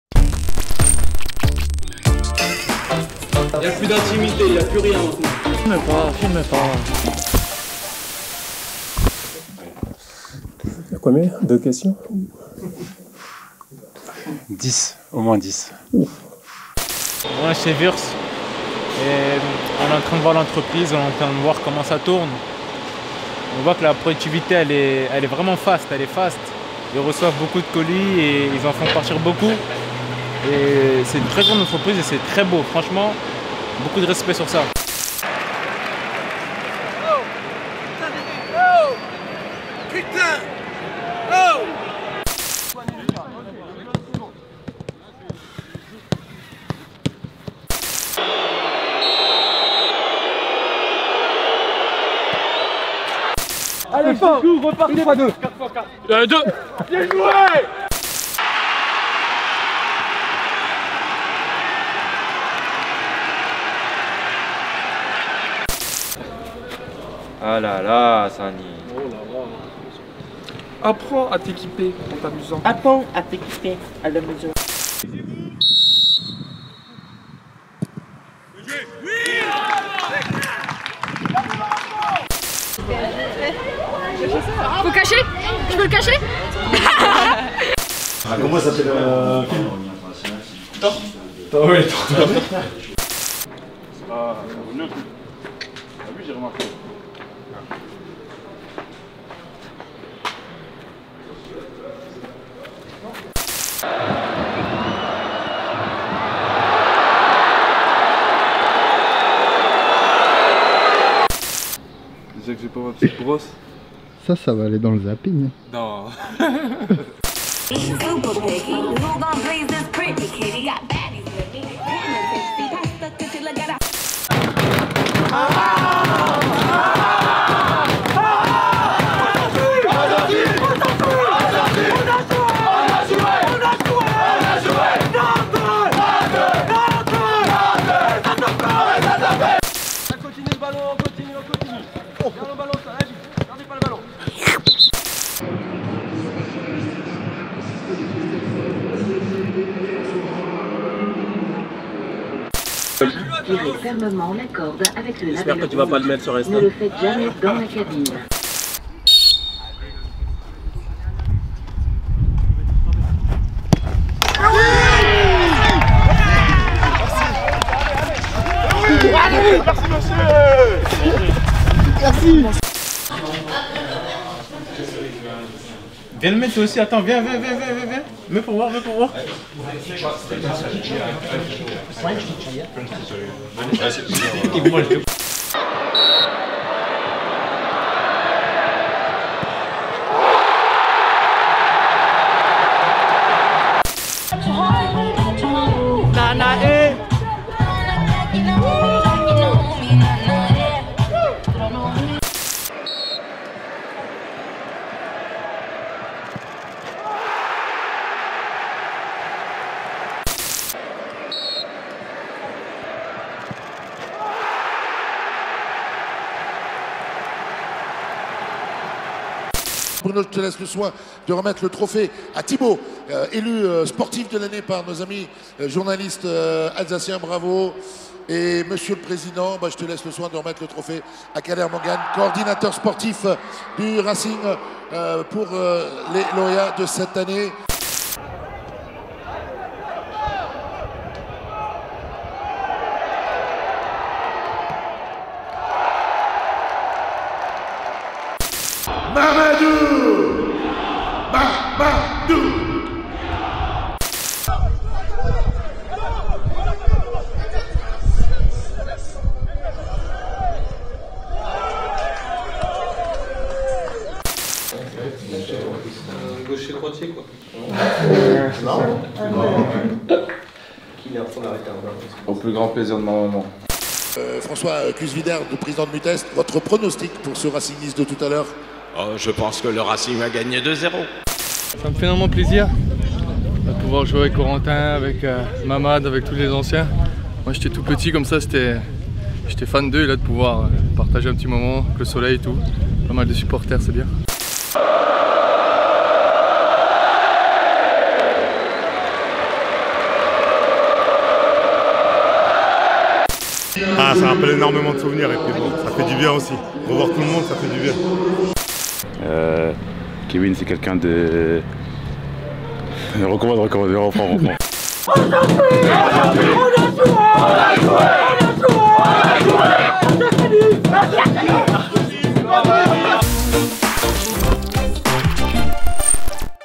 FILMEPASÇA - Episode 14
C'est parti pour les coulisses des dernières semaines au Racing ! Au programme de ce zap : le passage de nos Féminines à Shopping Promenade, Jeanjean Bellegarde, Eiji Kawashima et Colin Dagba en admiration chez Würth, les buts en bord terrain, les petits ratés de nos jeunes joueurs, etc.